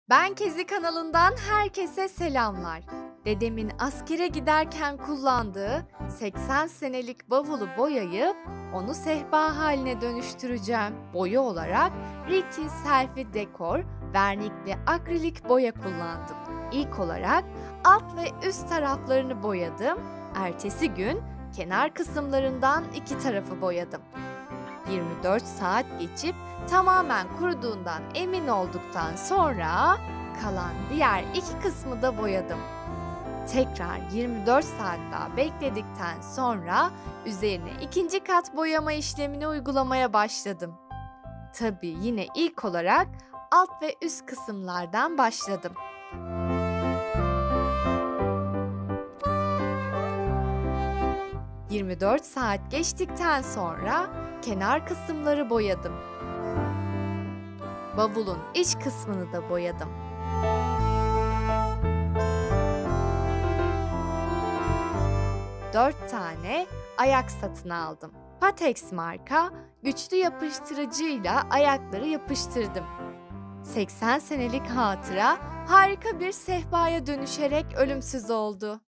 Eski bavuldan sehpa yapımı - Eski bavulu değerlendirme - Eski bavuldan neler yapılır?
Dedemin askere giderken kullandığı 80 yıllık bavuldan sehpa yaptım. Eski eşyaları atmayıp geri dönüşüm projesi olarak değerlendirmek bana mutluluk veriyor. Eski bavulla neler yapılır diye düşünürken aklıma sehpa yapmak geldi. Sonra da  aklıma bu eski bavuldan makyaj konsolu olarak değerlendirmek geldi. Boyadığım eski bavulun içine makyaj malzemelerimi koydum. Bavulun açtığım kapak kısmına ayna yapıştırmayı düşünüyorum. Eski bavulun üst kısmına da yaza doğru bir yenileme yapacağım böylece çok daha harika bir görüntüye kavuşacak. Yaza doğru yapacağım bu bavul yenileme videosunu da yükleyeceğim. Eski bavul boyama ilk başta beni korkuttu çünkü ilk kez boyama yaptım ama sonucu korktuğum gibi olmadı. Son derece şık bir bavul sehpam olmuş oldu. Evinizde bulunan eski bavulu bu şekilde değerlendirebilirsiniz.